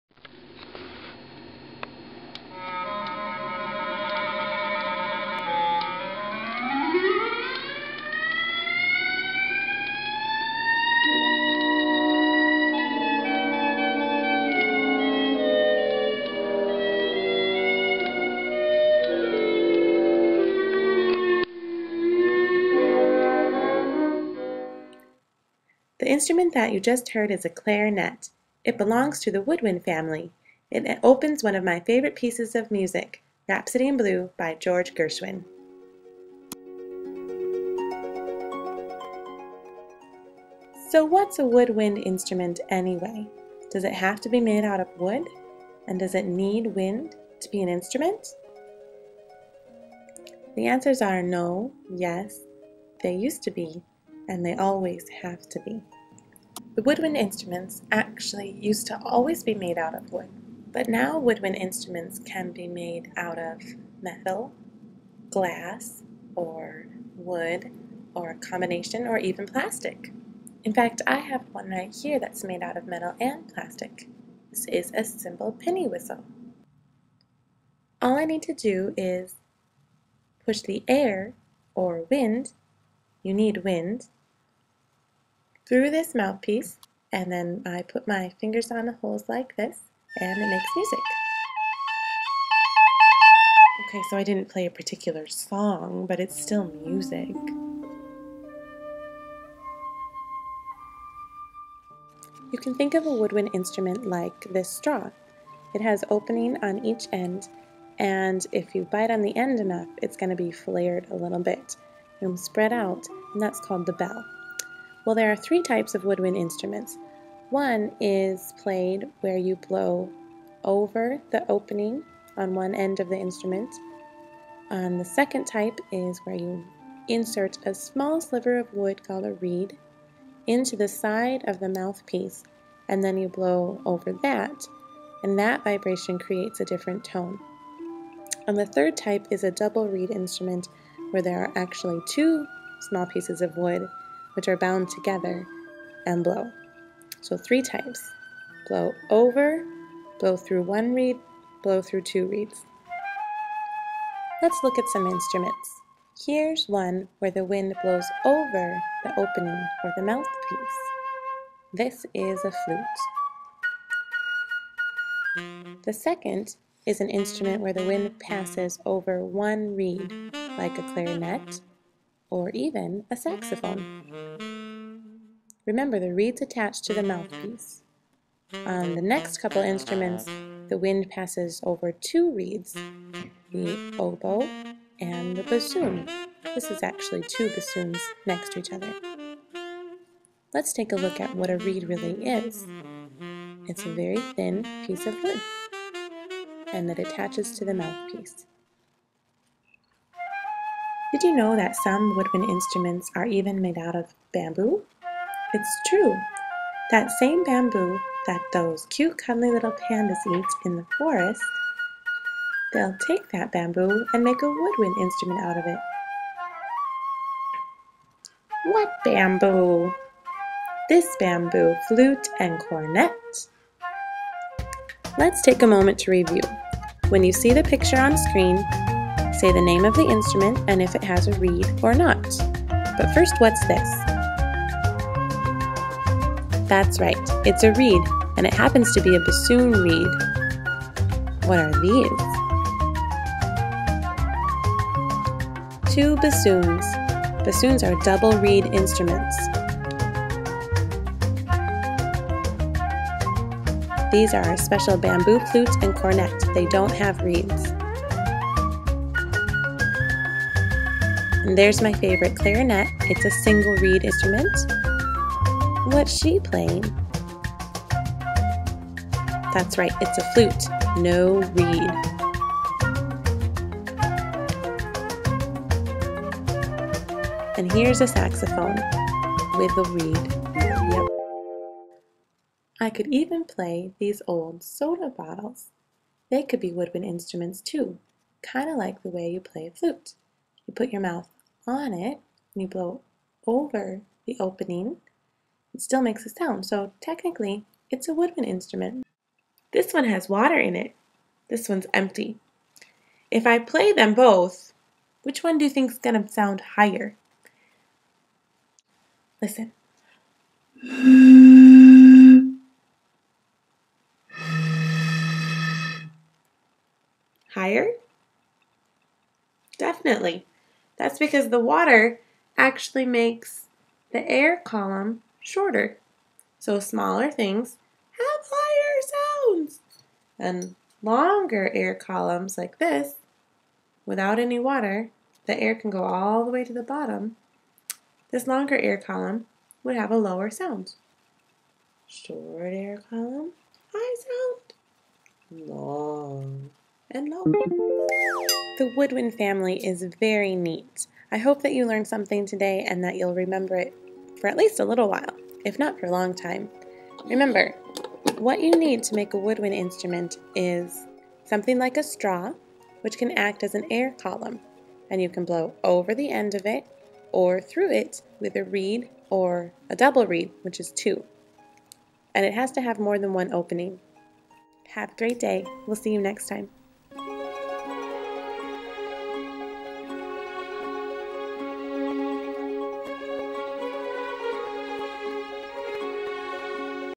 Woodwinds
Sommer Robles teaches elementary music in Northern Minnesota.  She is currently studied for a master's degree in education through Bemidji State University with a focus on arts in education. studied music composition and communications at Wheaton College in Wheaton, IL.  This is part three in a series of three.